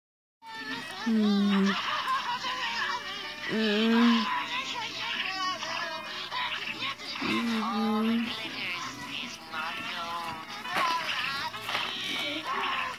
My reaction to Danny Phantom got cancelled in 2007
I need to bring Danny Phantom back